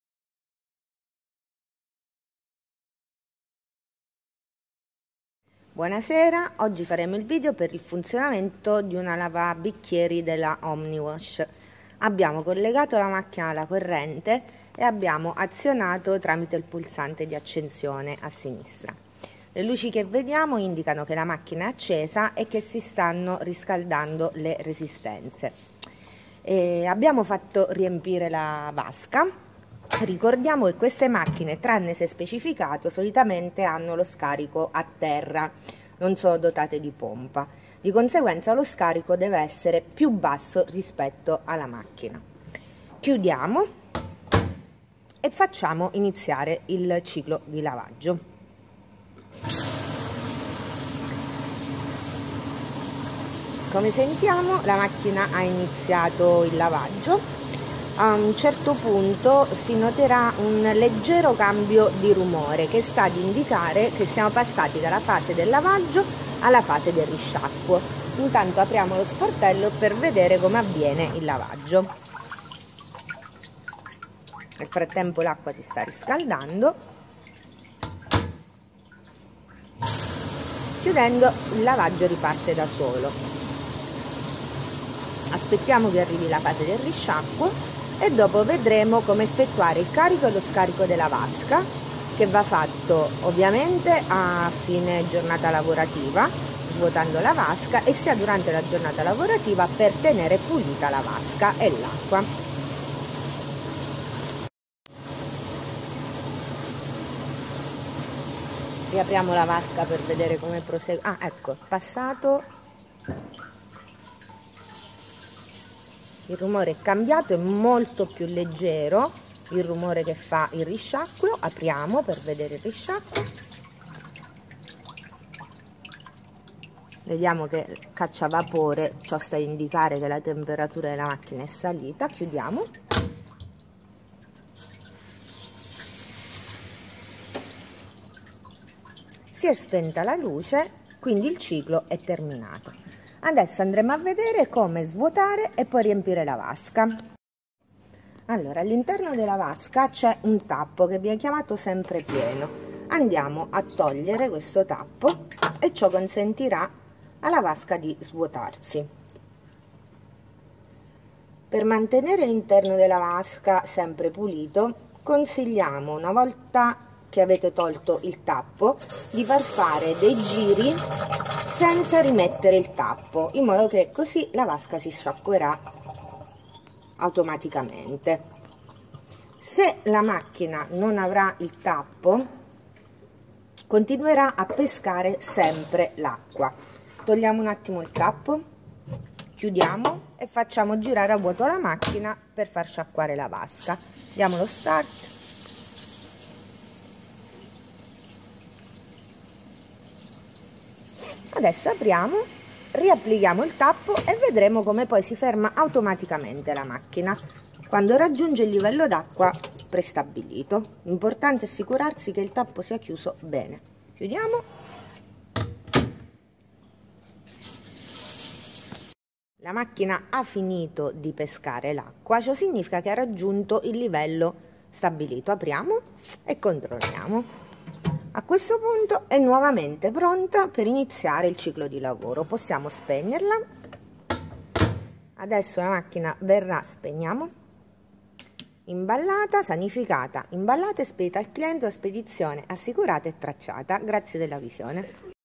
Lavabiccheri Omniwash - istruzioni per l'uso
Tutorial per l'utilizzo di una lavabiccheri Omniwash, cesto qadro.
Carico della vasca, scarico della vasca, cambio dell'acqua, lavaggio e risciacquo.
Teknoccasioni vende macchinari per: bar, ristorante, pasticceria, gelateria, pizzeria, gastronomia, e pubblici esercizi in genere.